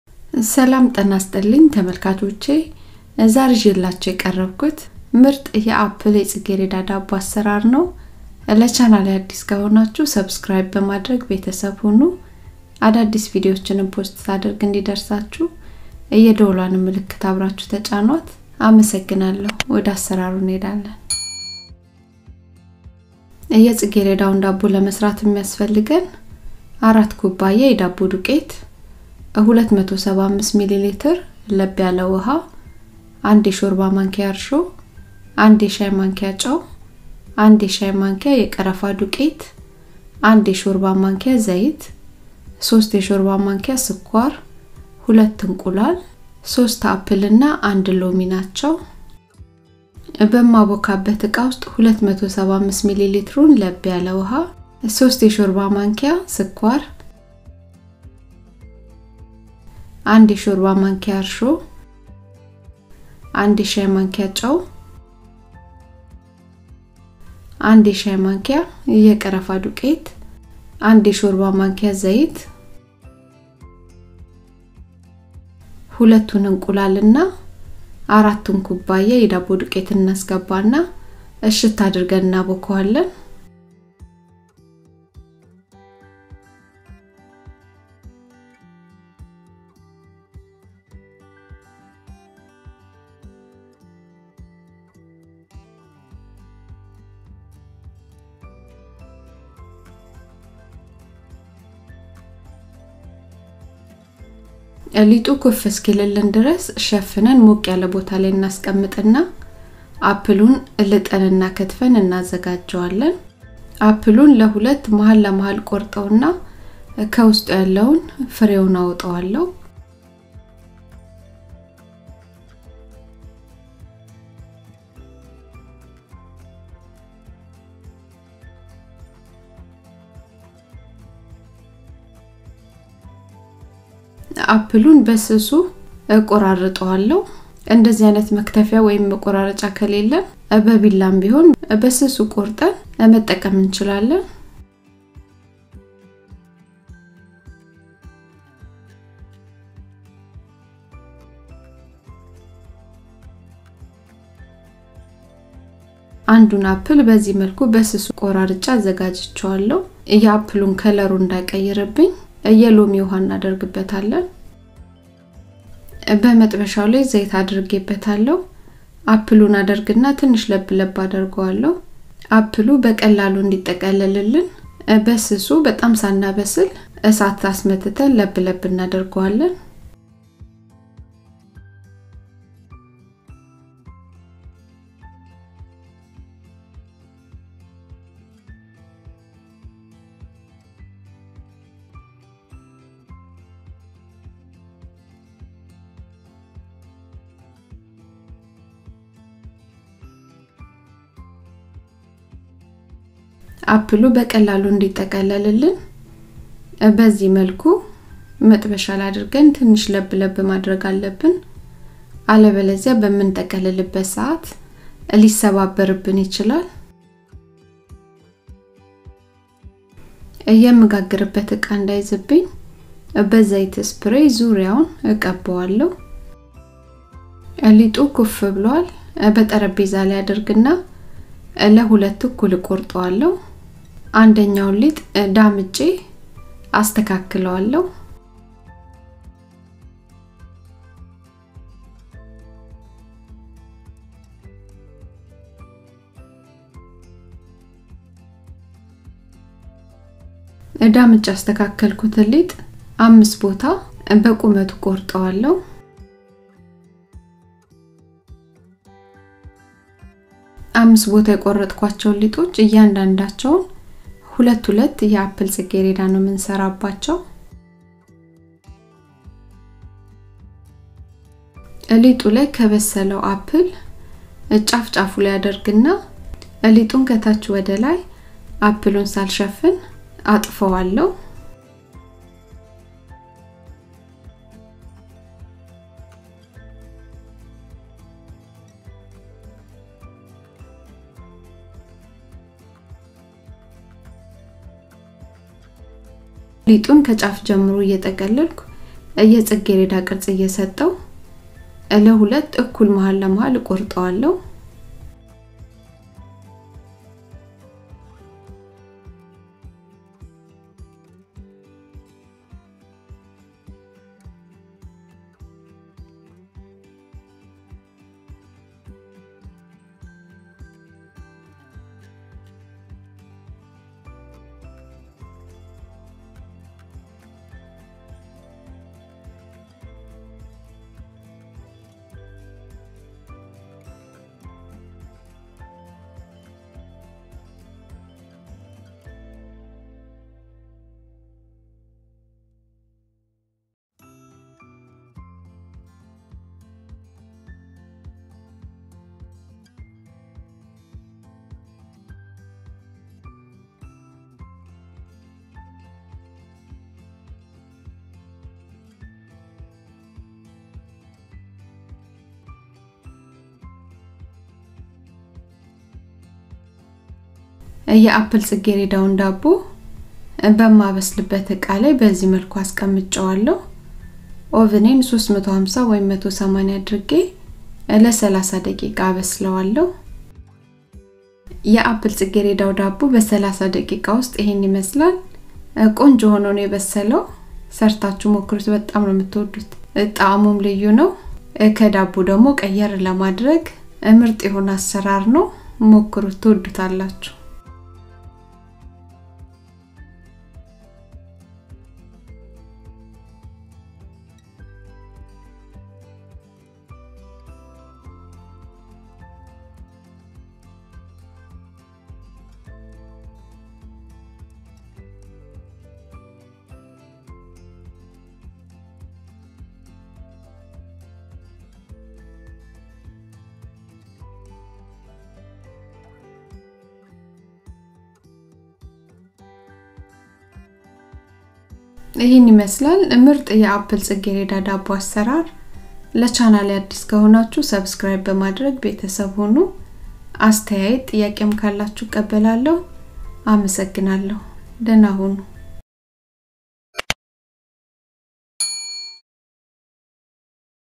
ልዩ አሰራር የአፕል ፅጌረዳ ዳቦ
. በመጥበሻ የተጋገረ ምርጥ የአፕል ኬክ አሰራር

EthioTastyFood